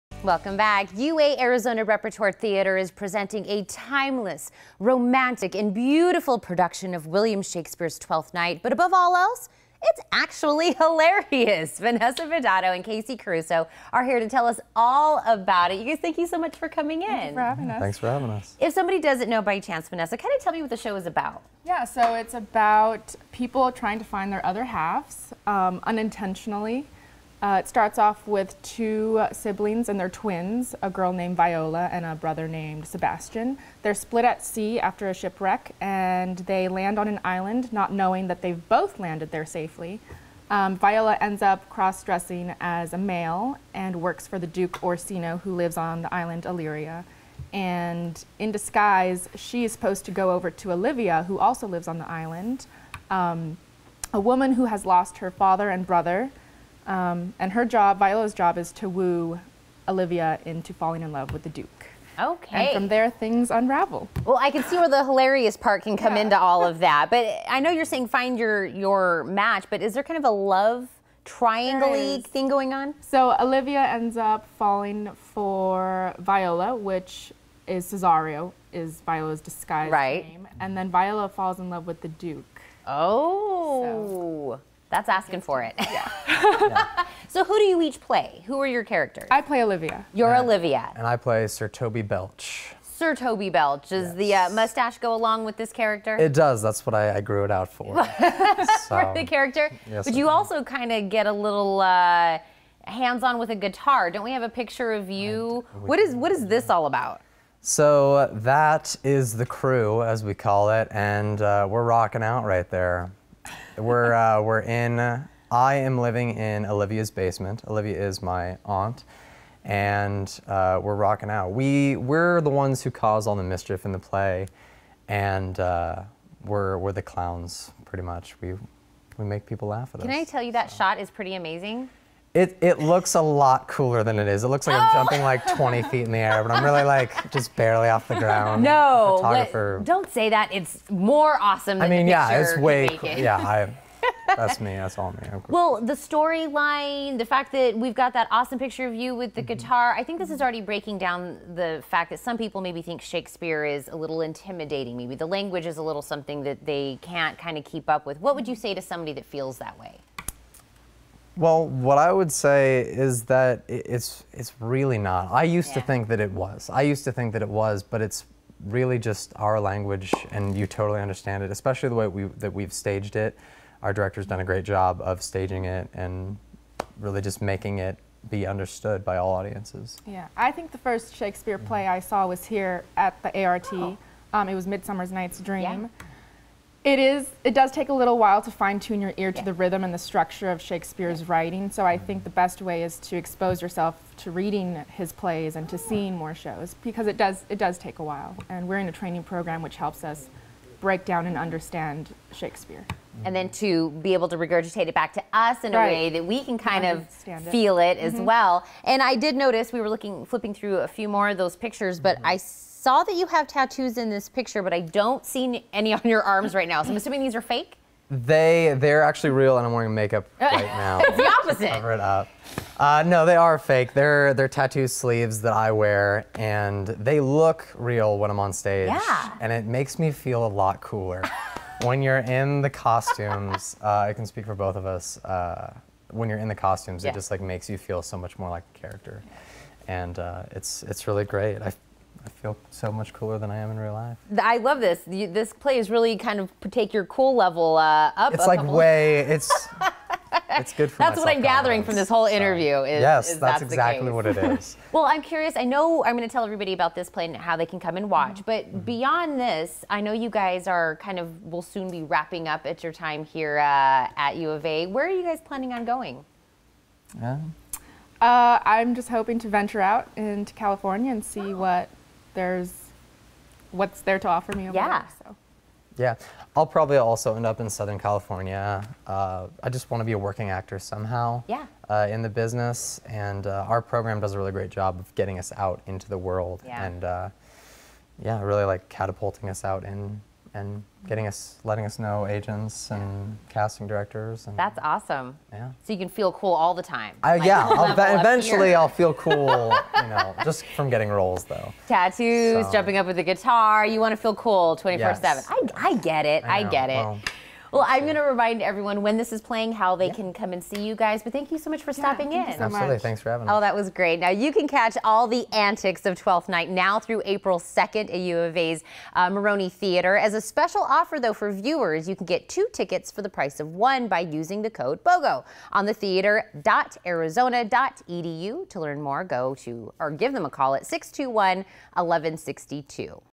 Arizona Repertory Theatre: Twelfth Night
UA Arizona Repertory Theatre is presenting a timeless, romantic and beautiful production of William Shakespeare’s TWELFTH NIGHT, but above all, it’s HILARIOUS! See Tomorrow’s Stars Today at UA Arizona Repertory Theatre…the OTHER Wildcat sport!